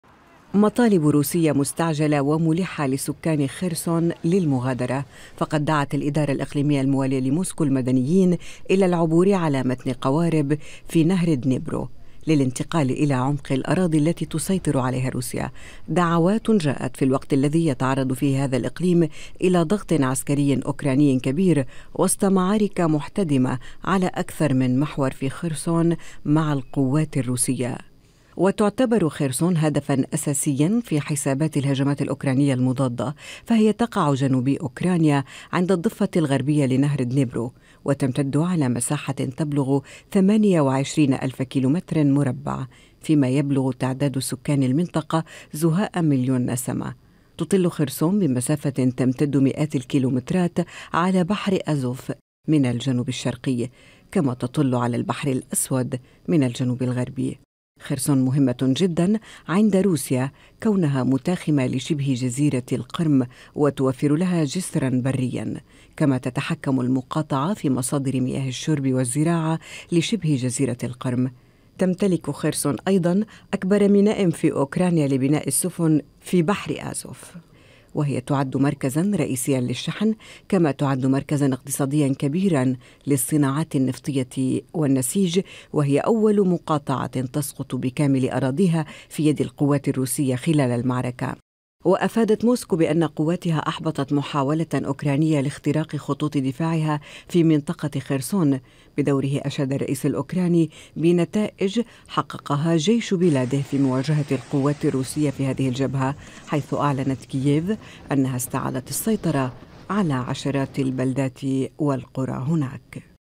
أزمة أوكرانيا..سلطات خيرسون تطلب من المدنيين المغادرة بشكل فوري
هذا ودعت السلطات الموالية لموسكو في خيرسون جنوبي أوكرانيا، جميع السكان إلى الإخلاء الفوري والعبور على متن قوارب في نهر الدنيبرو إلى عمق الأراضي التي تسيطر عليها روسيا.
تأتي هذه التطورات في وقت تتواصل فيه المعارك بين القوات الروسية والقوات الأوكرانية التي تشن هجمات من أجل السيطرة على خيرسون.